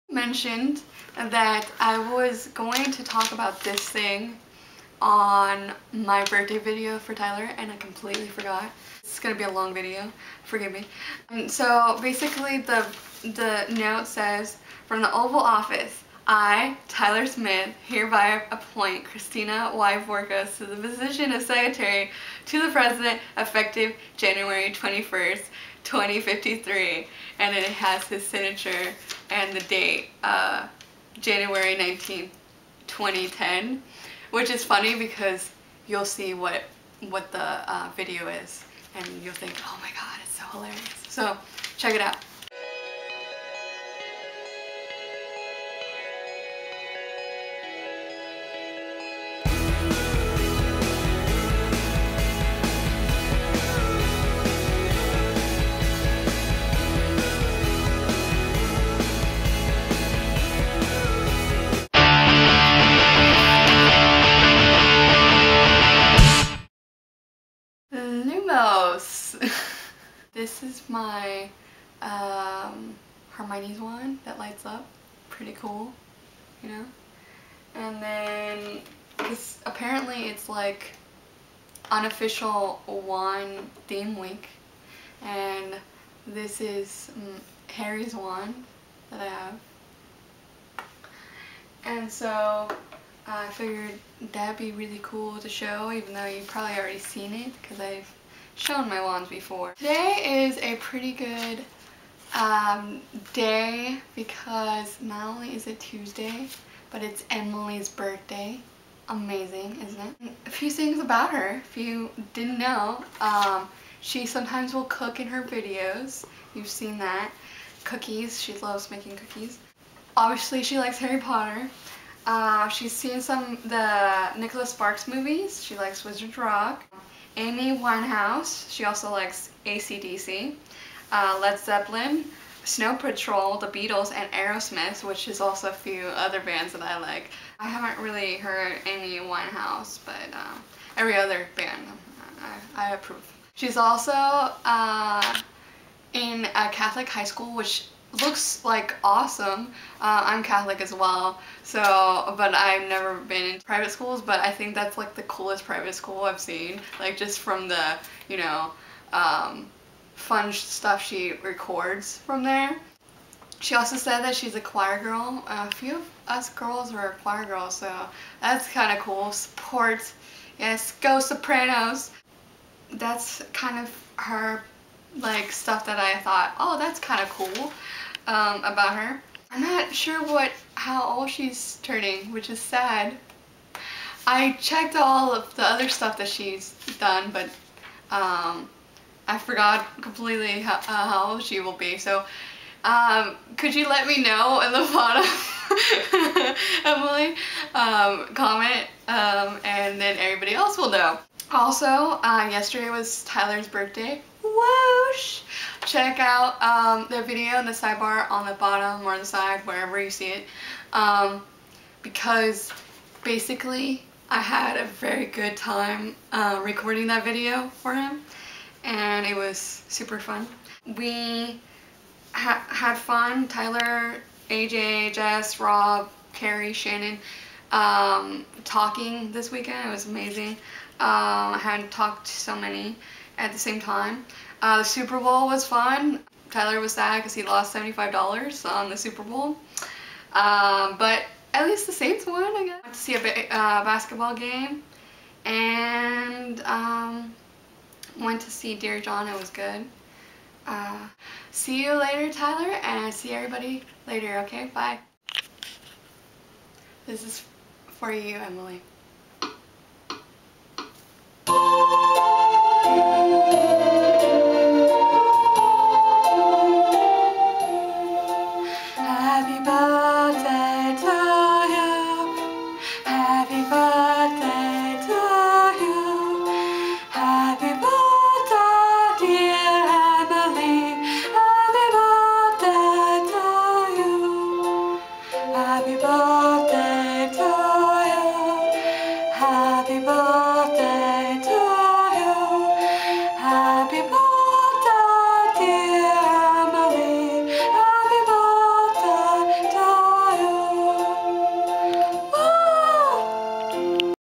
Tina Tuesdays - (2/9/10) It's a great day, it's Emily's Birthday!
Happy Birthday, Emily Schmidt!

It's been a great day! I already had my first class Shakespeare, later tonight I'll have Into to Linguistics. This weekend I took some pictures of my cousins but two of them made me smile. While I was walking earlier on campus I saw our native wild cats and took pictures of them by the Faculty Center. Then at lunch I decided to take a picture of my green to-go containers for Jen. She's a HP fan that is in college too and she was curious about them. They had cake so I couldn't pass up. Then later there was a college organization supporting 'Relay for Life' Cancer Society. I saw that and I had to help out. I might go at night to the university on thursday for "Speed Dating", some group on campus is doing it for the singles on campus for Valentine's Day. I've already signed up but I don't know if I'm going to go. It sounds like it's going to be fun.

Thanks to Rob for doing a great job on his video for Jess on monday!! He worked hard on it so I hope everyone liked it.

The Preview will be on Domingo Live this Sunday at 10:30 am. Guess who's recording? haha!

Much love,
Tina